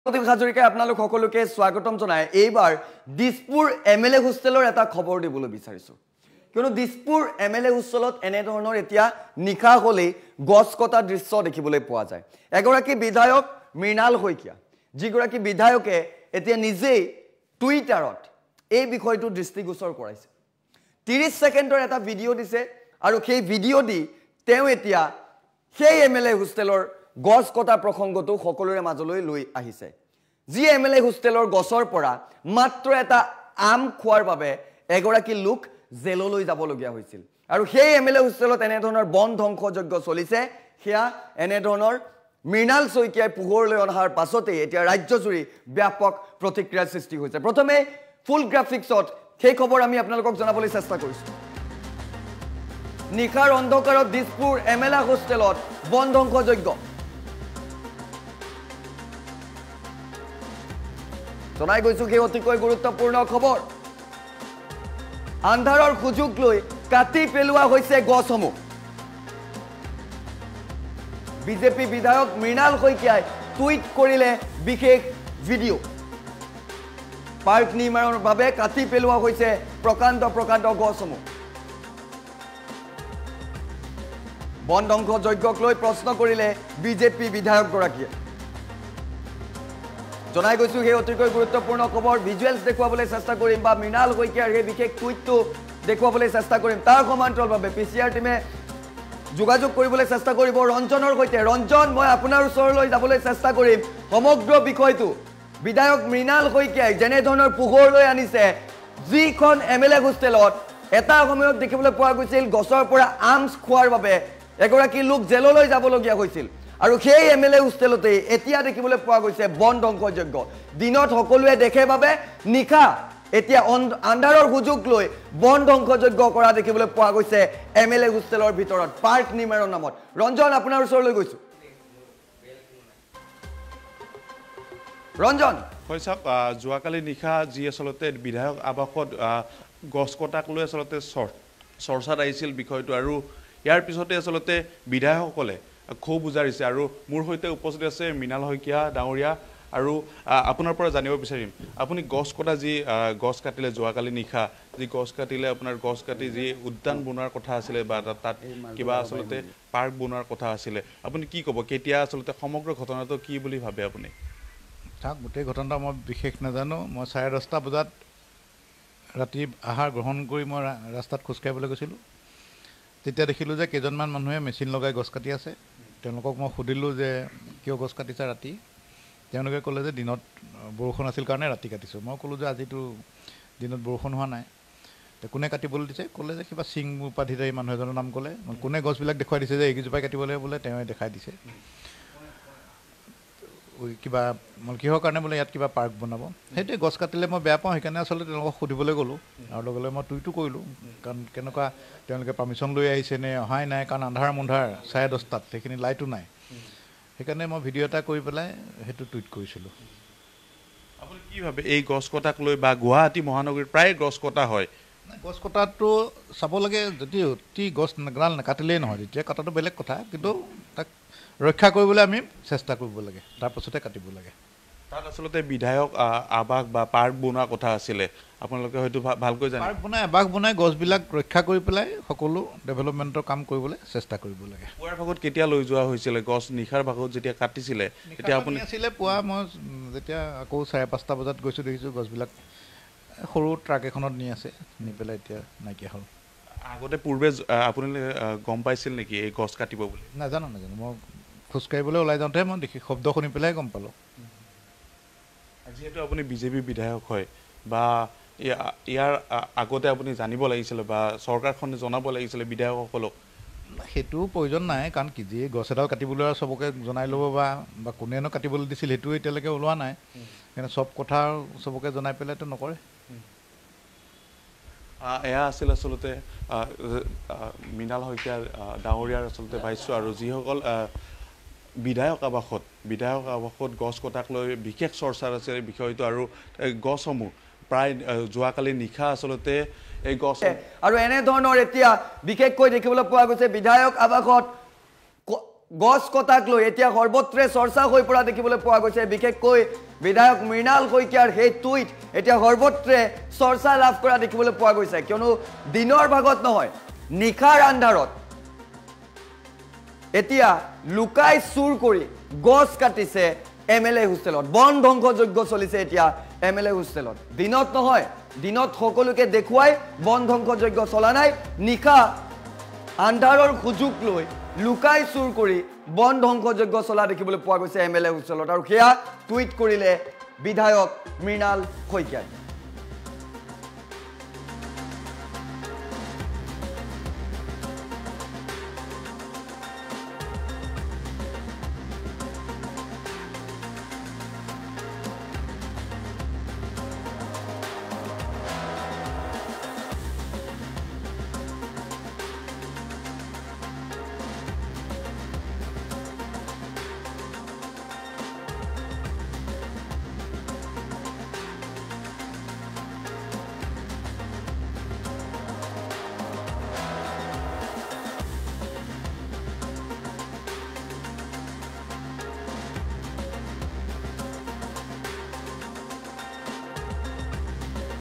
নিশাৰ অন্ধকাৰত দিশপুৰৰ MLA হোষ্টেলত বন ধ্বংসযজ্ঞ।
নিশাৰ অন্ধকাৰত দিশপুৰৰ MLA হোষ্টেলত বন ধ্বংসযজ্ঞ। বিধায়ক আৱাসত কটা হৈছে প্ৰকাণ্ড প্ৰকাণ্ড গছ।